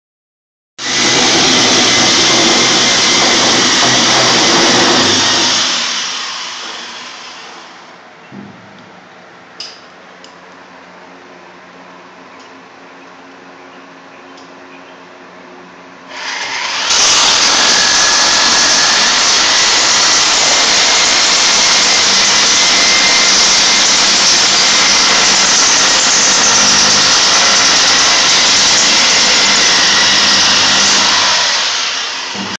Tornado Alley Armor- drilling holes for safe room wedge anchors
This short video shows how easy it is to drill holes for Tornado Alley Armor wedge anchors using a rotary hammer. Rotary hammers are worth every penny. They actually have a little internal piston that hammers down on the bit, making it a little rotating jackhammer. A standard hammer drill would take considerably longer. You can rent a rotary hammer pretty cheaply for a day from most rental outlets.